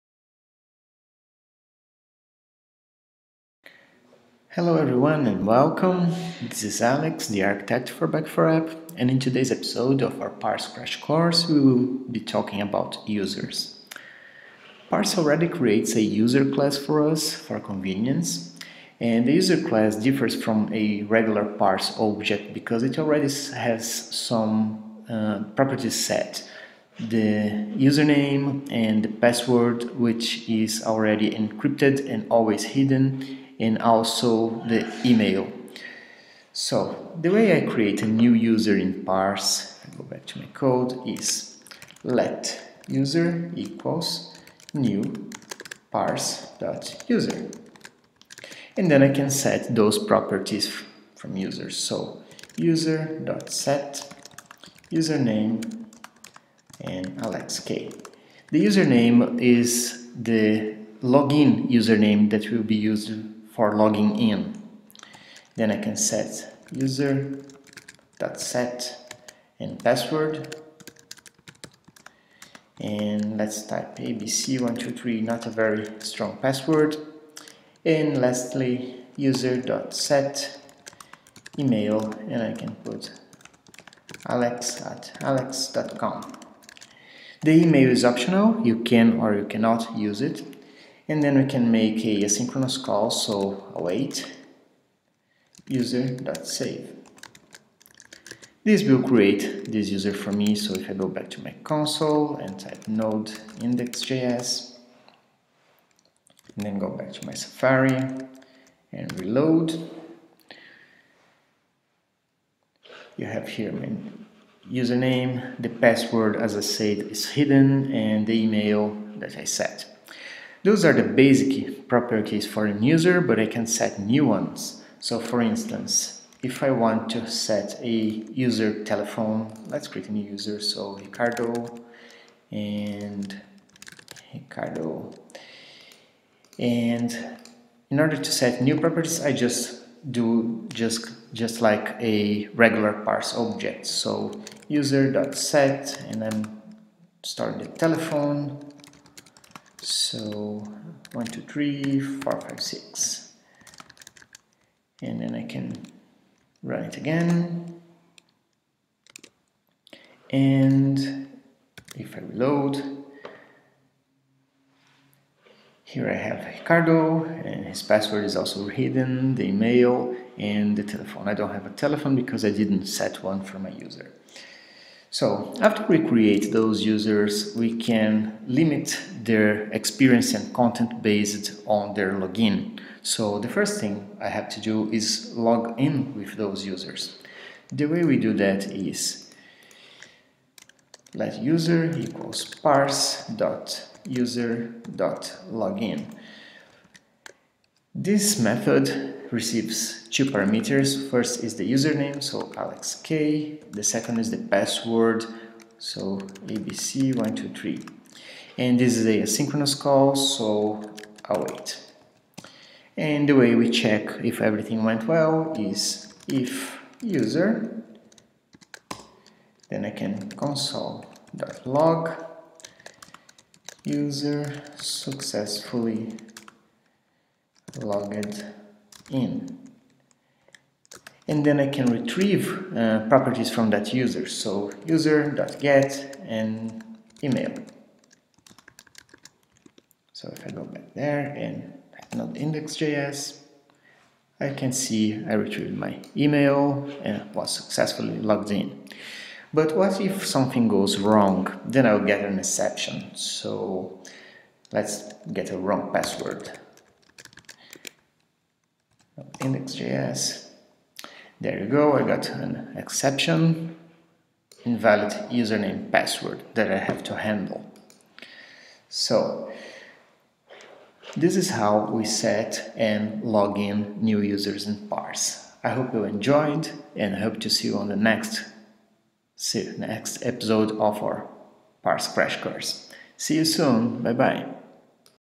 Parse Crash Course #10 - Using the default Parse User Class to manage users
In this episode, Alex K. will show you how to create and manipulate users on default Parse User Class

Release your app faster and better, sign up to Back4App, the complete open source serverless backend platform.